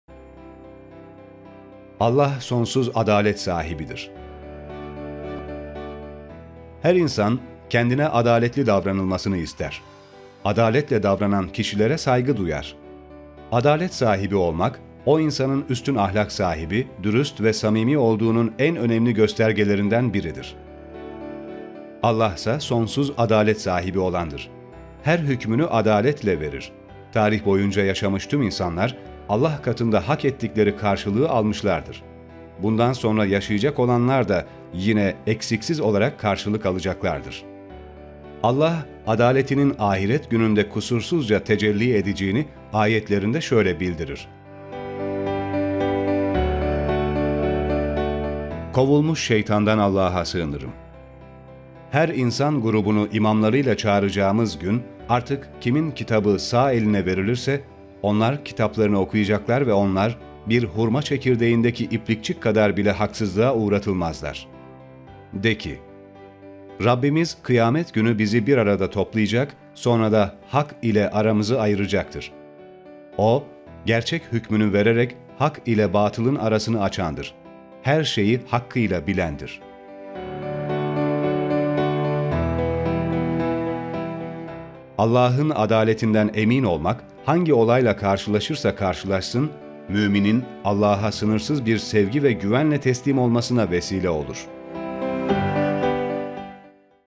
Allah Sonsuz Adalet Sahibidir
Her insan kendine adaletli davranılmasını ister. Adaletle davranan kişilere saygı duyar. Adalet sahibi olmak, o insanın üstün ahlak sahibi, dürüst ve samimi olduğunun en önemli göstergelerinden biridir. Allah ise sonsuz adalet sahibi olandır. Her hükmünü adaletle verir. Tarih boyunca yaşamış tüm insanlar Allah katında hak ettikleri karşılığı almışlardır. Bundan sonra yaşayacak olanlar da yine eksiksiz olarak karşılık alacaklardır. Allah adaletinin ahiret gününde kusursuzca tecelli edeceğini, ayetlerinde şöyle bildirir:

“Her insan grubunu imamlarıyla çağıracağımız gün, artık kimin kitabı sağ eline verilirse, onlar kitaplarını okuyacaklar ve onlar, bir 'hurma çekirdeğindeki iplikçik kadar' bile haksızlığa uğratılmazlar.” (İsra Suresi, 71)

“De ki: “Rabbimiz (kıyamet günü) bizi birarada toplayacak, sonra da hak ile aramızı ayıracaktır. O, (gerçek hükmünü vererek hak ile batılın arasını) açandır, (her şeyi hakkıyla) bilendir.” (Sebe Suresi, 26)

 Allah'ın adaletinden emin olmak, hangi olayla karşılaşırsa karşılaşsın mümin'in Allah'a sınırsız bir sevgi ve güvenle teslim olmasına vesile olur.